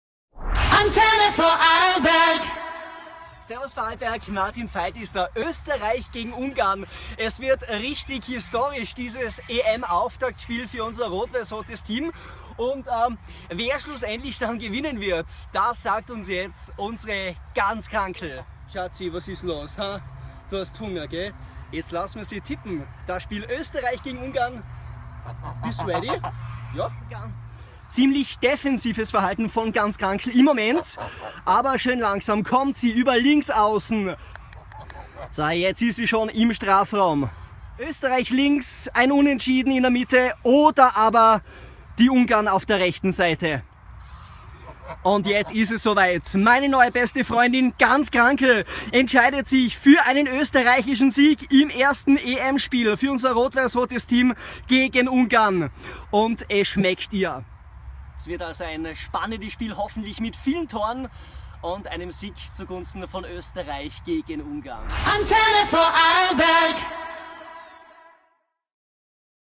Fußball-EM 2016: Unsere Gans Krankl tippt Österreich vs. Ungarn!
Wie geht das Spiel Österreich vs. Ungarn aus? Unsere Gans Krankl ist sich sicher: Österreich gewinnt!